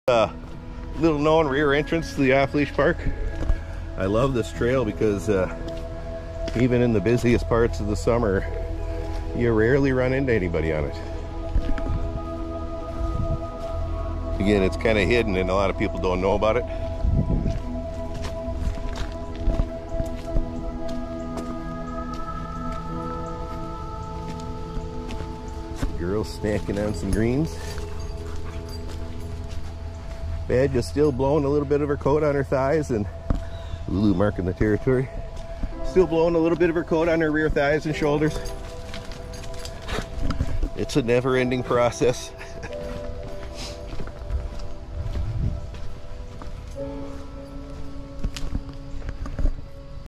SECRET Hidden Entrance into America's Best Dog Park
This is the 1st posted hiking video with Ulu since having her litter. We take her out with her older daughter Honey Badger. Watch them run a play at the huge wilderness off leash park in the Mississippi River valley in MN. on a rainy summer morning.

!!! 1st 508th Airborne TEESPRING MERCHANDISE...